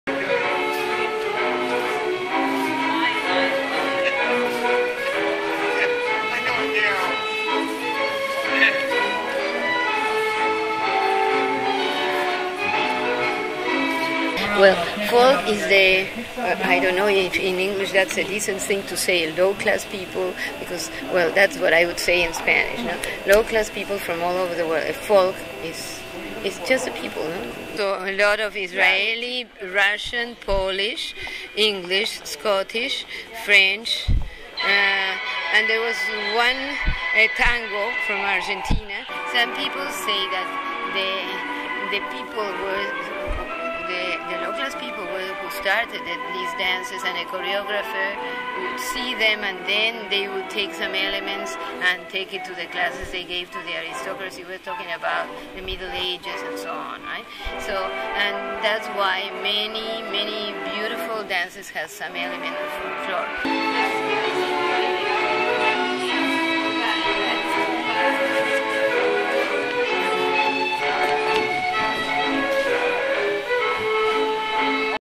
International dancers bring folk culture to Balboa Park
The International Folk Dancers get together every week in Balboa Park and listen to traditional folk music and perform international folk dances.

Dancer Nelly Philipps talks about the dances they do and some history of folk dance.